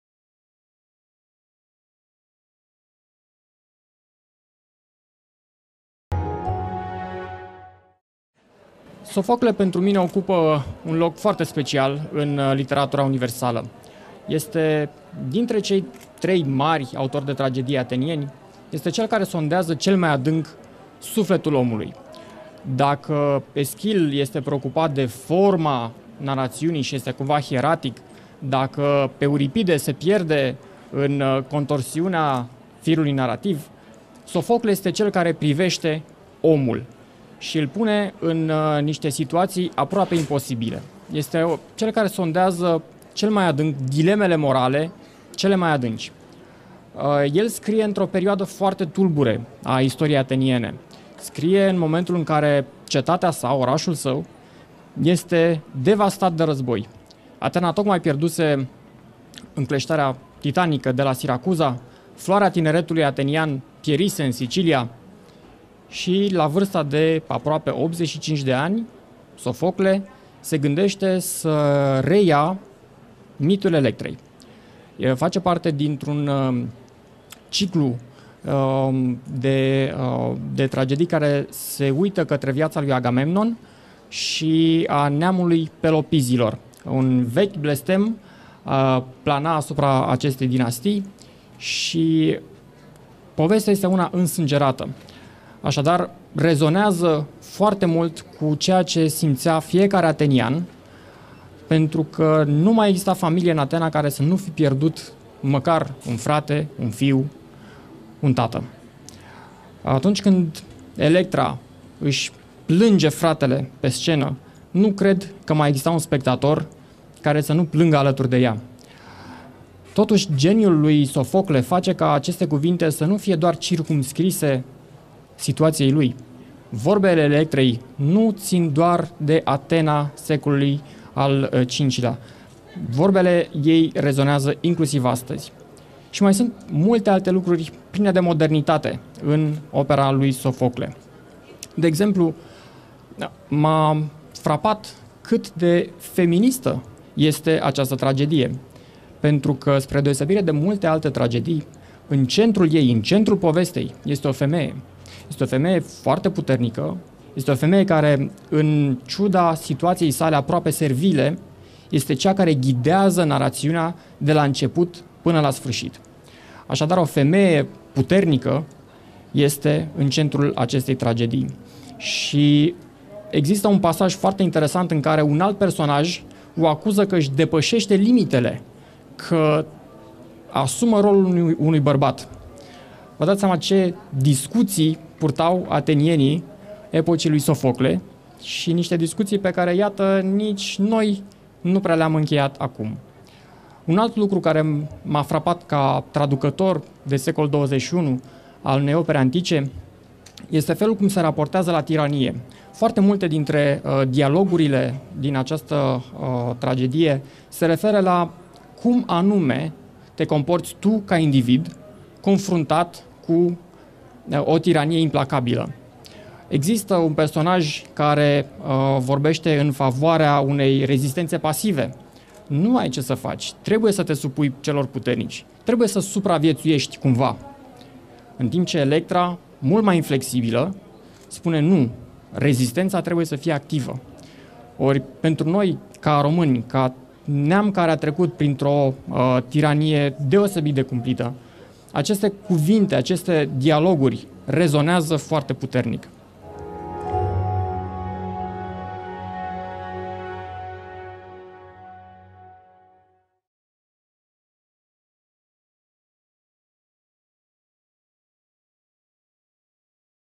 Icpedia - Editura Electra Ep. 3
Electra is one of the late pieces of Sophocles, most probably written around 410 BC. At this time, Athens was caught in the titanic clash of the Peloponnesian War (431–404 BC) against Sparta. In the midst of these troubled times, old Sophocles finds inspiration to write an extraordinary play about duty, justice, tyranny, family, and providential heroes. Of all the characters in the play, by far the most important is Electra. It is the driving force of the play, setting in motion and supporting the entire narrative thread. Electra is a force of nature - the violence with which she hates those who killed her father is matched only by the love she has for her brother in exile. However, he is a character who bears immense pain.